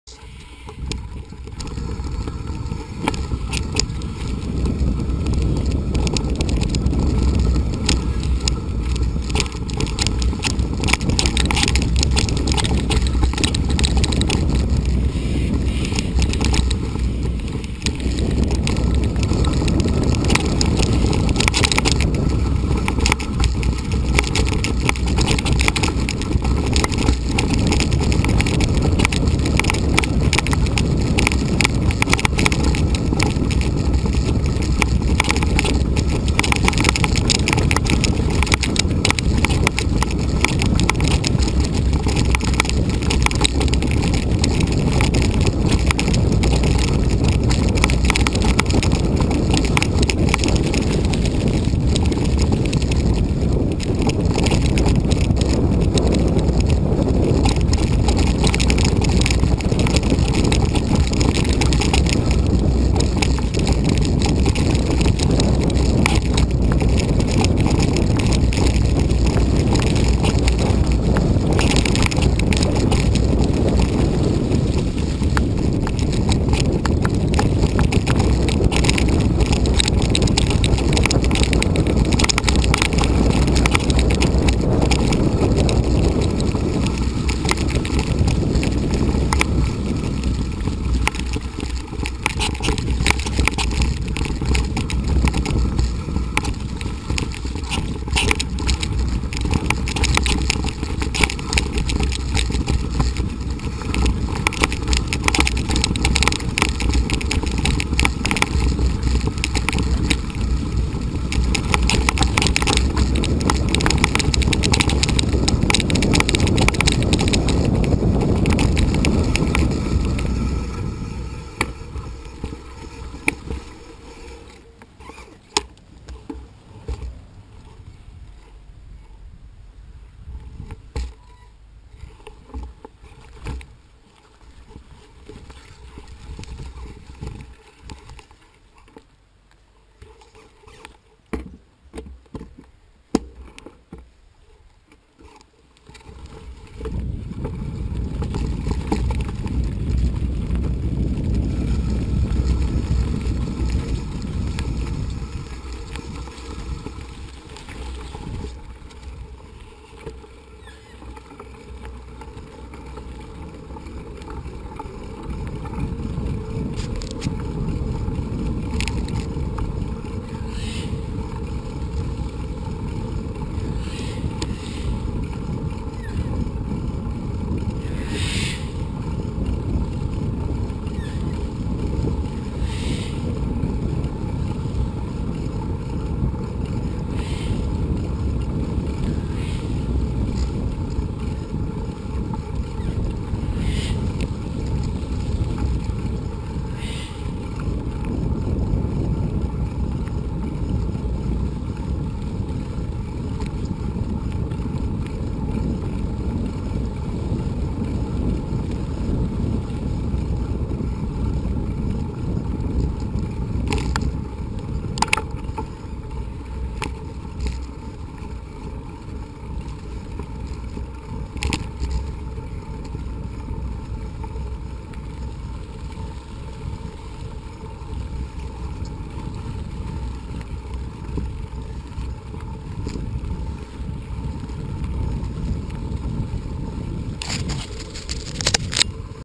Ion Air Pro - Dodds Track Doongalla Rd
The ride along Dodds Track to Doongalla Rd filmed using Ion Air Pro HD action sports video camera mounted on the handlebars.

Best viewed in 1080p HD Full-screen.